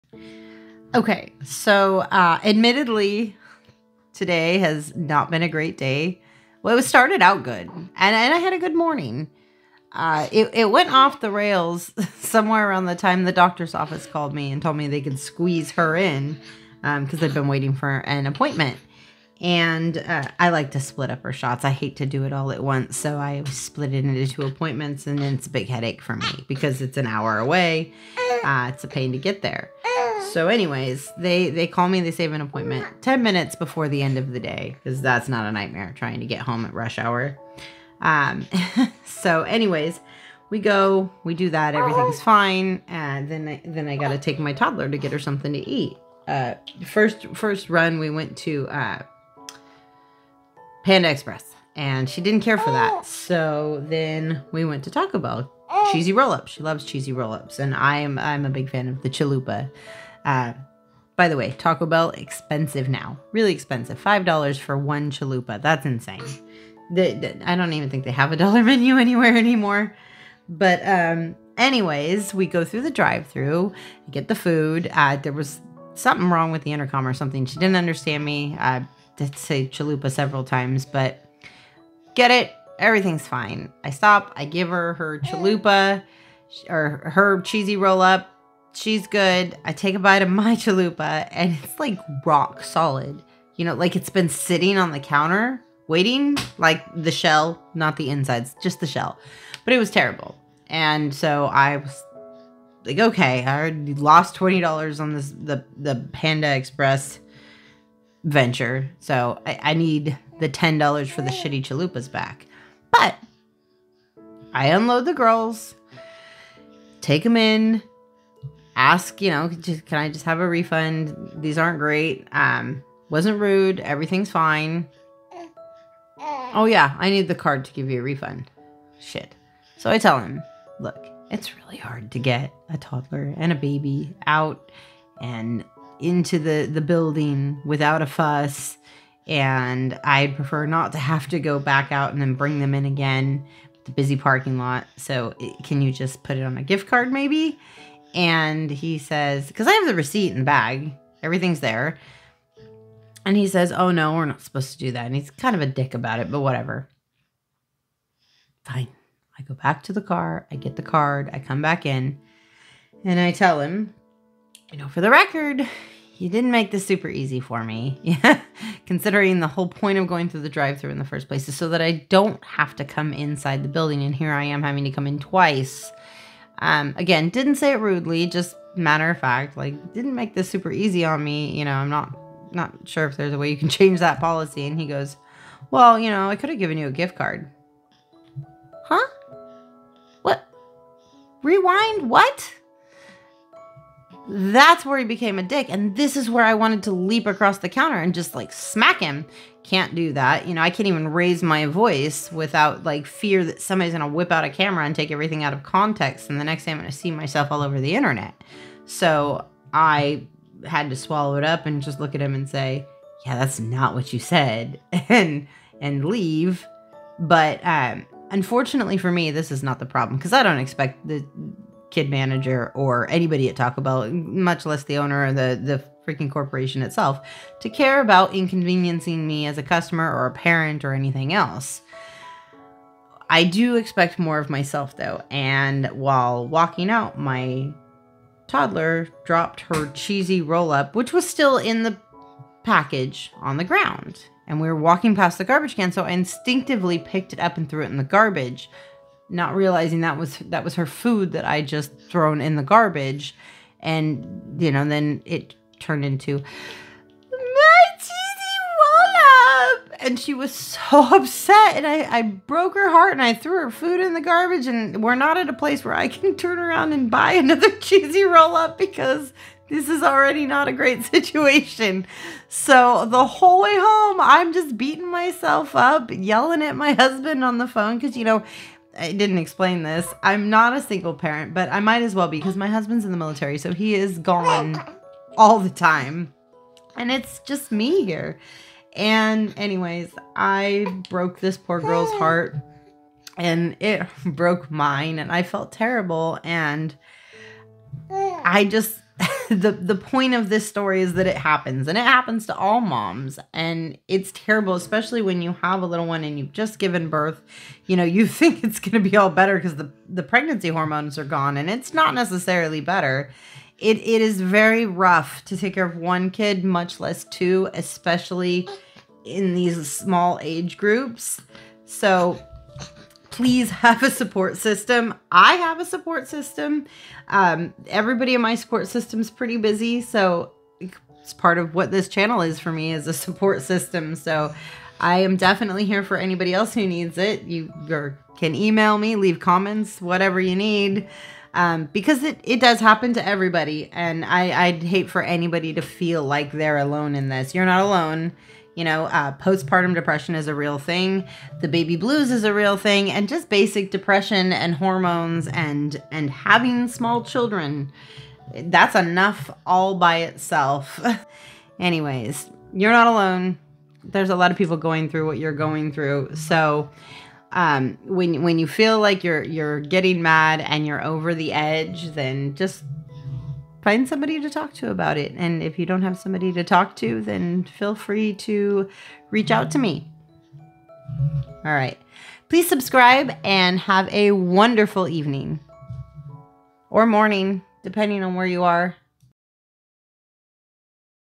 The Postpartum Struggle is Real
Unexpected breakdowns and how to deal when things go off the rails.